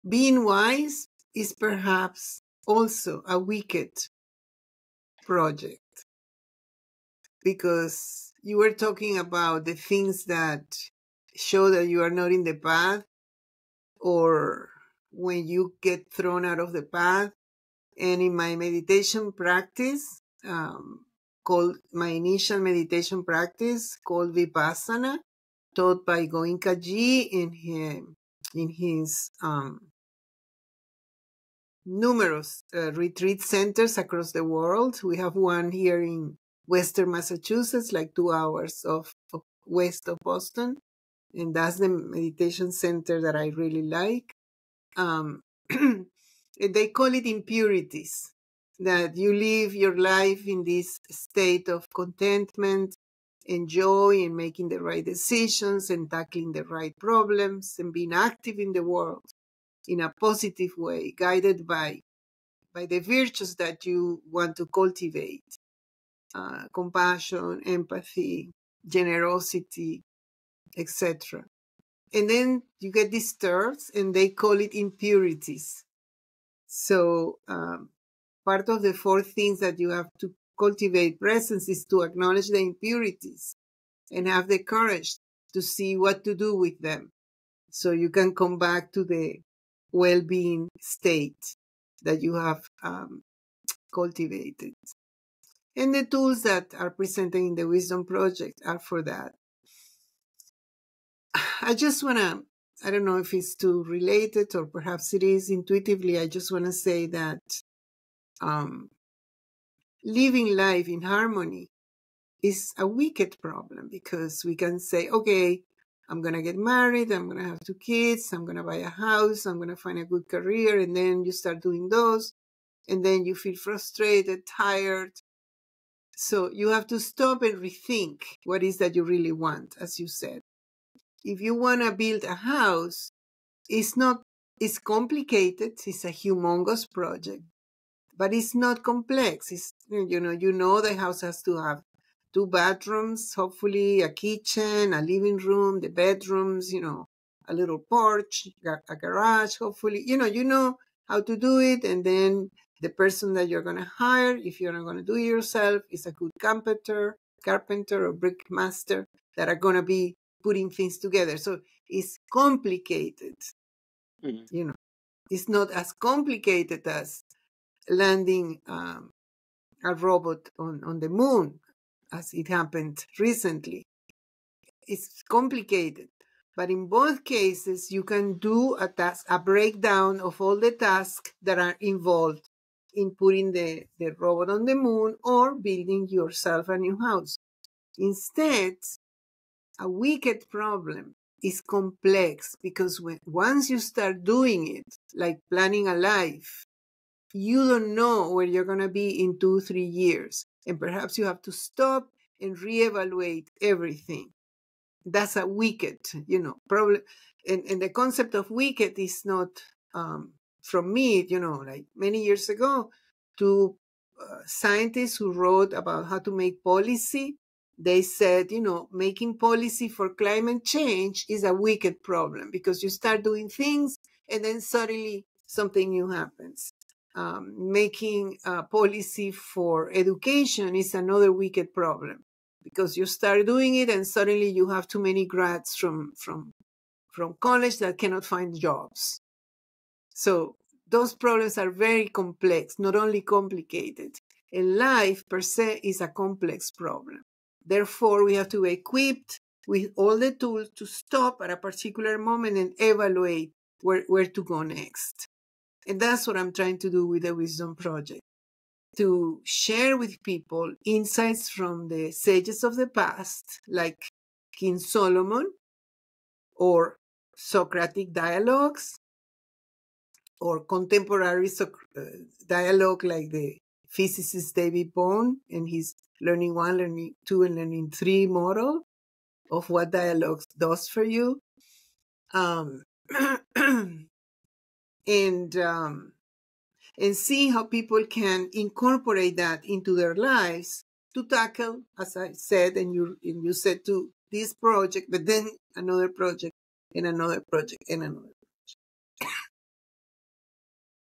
Navigating Life as a Wicked Problem
The conversation explores the concept of living life in harmony and the challenges it presents. It discusses the idea of impurities and how they disrupt the state of contentment and well-being. The WISDOM project is introduced as a tool to cultivate presence and address these impurities. The concept of wicked problems is also discussed, highlighting the complexity of planning and navigating life. The conversation emphasizes the importance of evaluating and reevaluating one's goals and direction. The Wisdom Project aims to provide insights from past sages and contemporary dialogues to help individuals tackle the various projects in their lives.